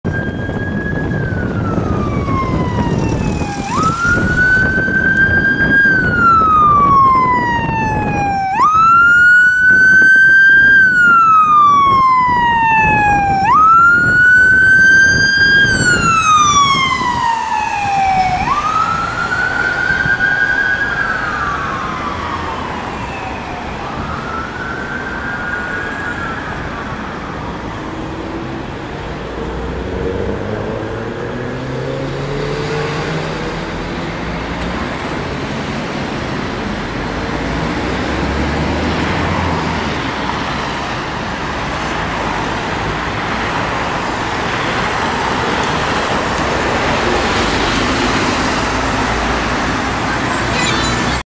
Prague Police MVA van responding | Praha Policie ČR VW T6 [CZ | 29.5.2019]
A Prague Police (Czech state Police/ Policie ČR) traffic/ MVA (motor vehicle accident) van responding. How do you guys like the rear graphics?

Prague, Czechia/ Czech Republic, May 29, 2019.